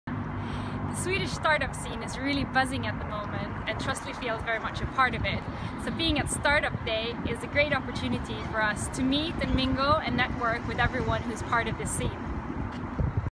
Startup Day 2015 Stockholm
Catherine Rhenberg - Head of marketplaces, short comment about the Swedish startup scene.